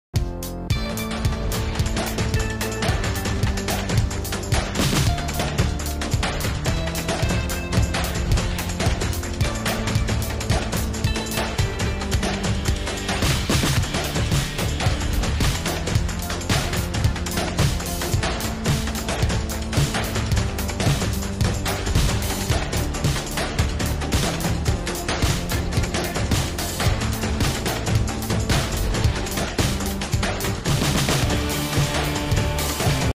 Australian cricketer Peter siddle fire power in pace bowling
Peter siddle Australian speedo star.
NOTE : This video isn't my property, The owner of this video is someone else.